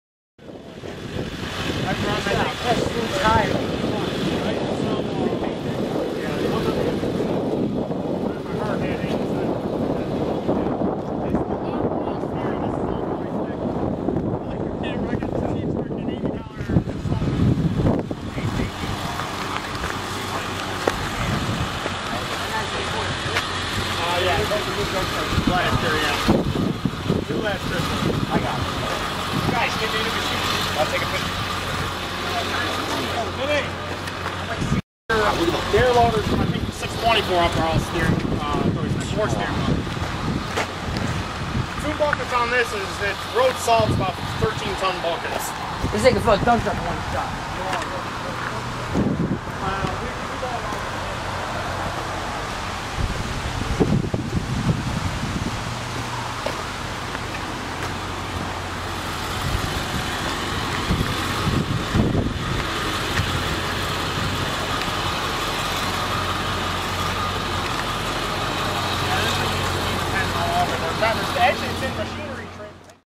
CAT 988F
A cat 988f in Ohio at a recycleing plant/quarry. it started raining at the end of the video, you can actually see the rain drops on the bucket. I would have got a video climbing on it but i didnt want my camcorder to get all full of water and break. Father back into the site there was a cat 320cl with a hammer and rock crushers screners and much more